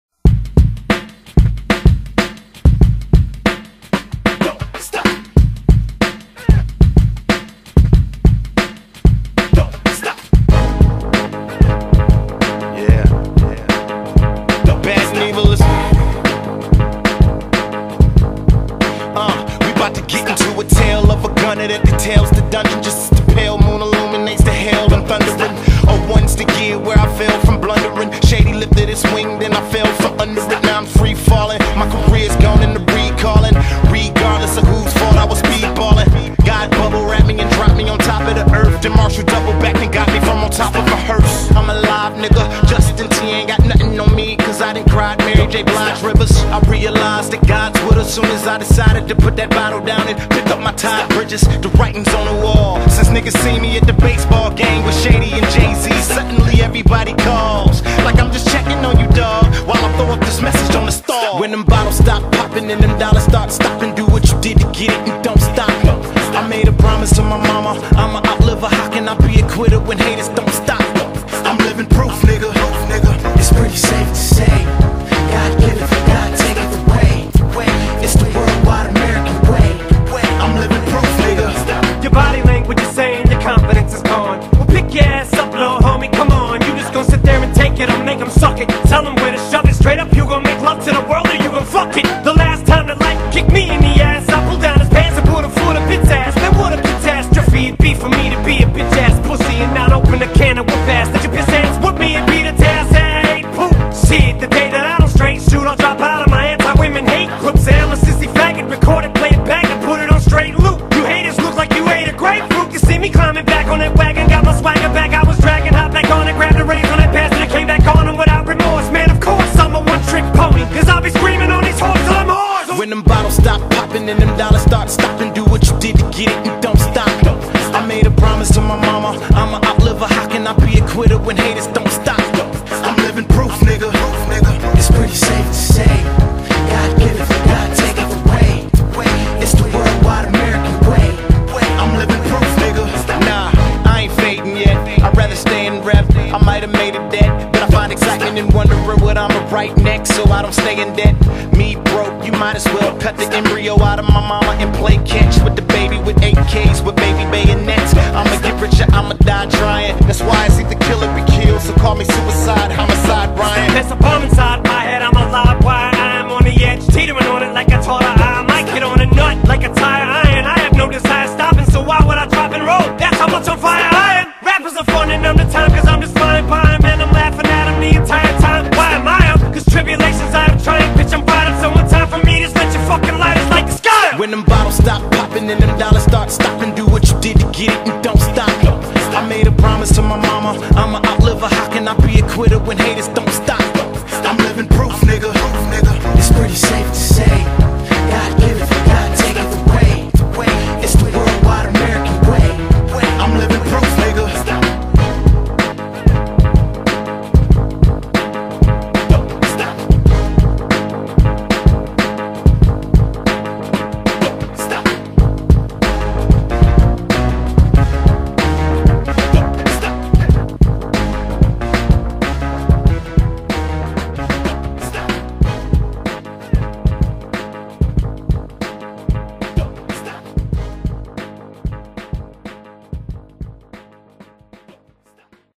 ReX vs RipZ 5-3 (Mad Haters)
Ok guys so dis is the second game ive done in about 3-4 weeks and i didnt do to hot but my nigga gunnaman went in with 13 heads while smokeyz the confused redneck who thinks hes black was flappin his lips so i hope you enjoy =)